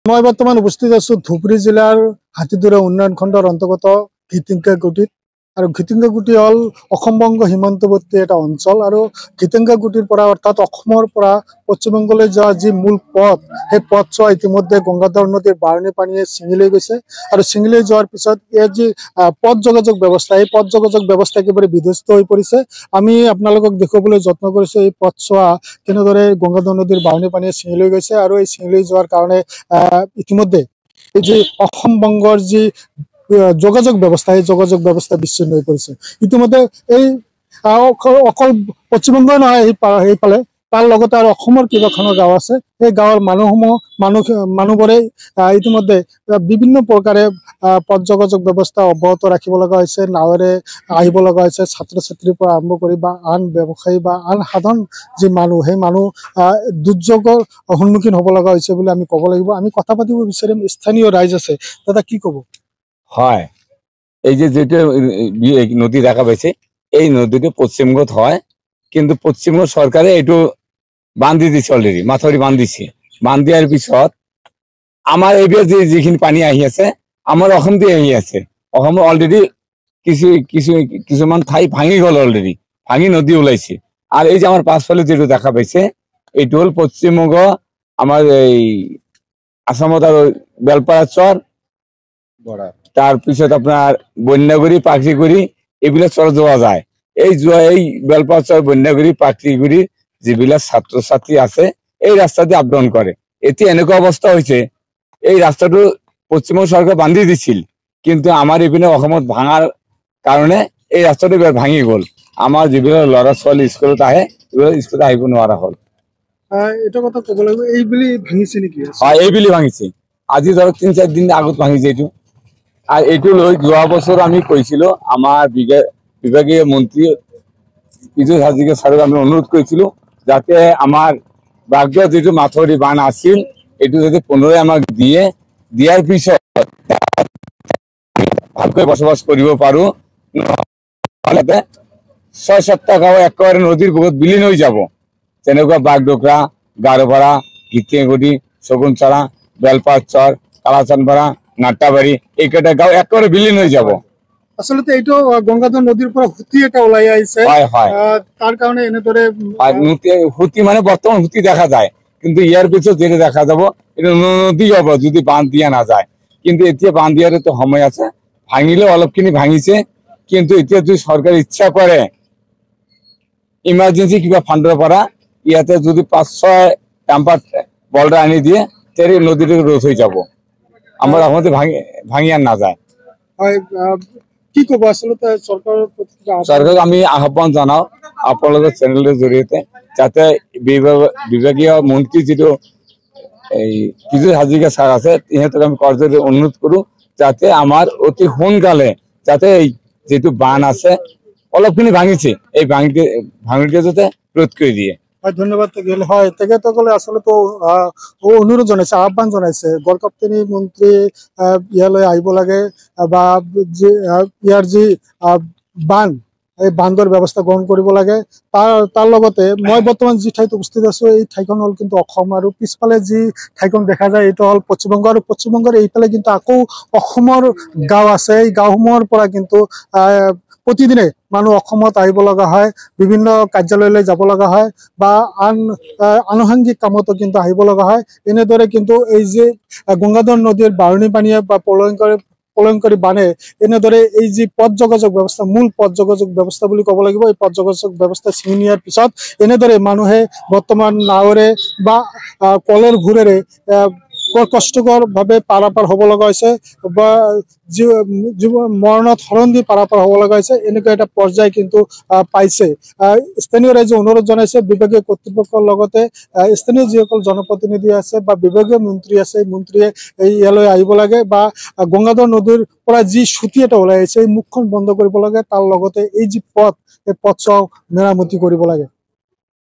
Gangadhar River : বাঢ়নি পানীয়ে ছিঙি নিলে অসম বংগ যোগাযোগৰ মূল পথ | N18V
Gangadhar River | গংগাধৰ নদীৰ বাঢ়নি পানীয়ে ছিঙি নিলে অসম বংগ যোগাযোগৰ মূল পথ । যাতায়ত বিচ্ছিন্ন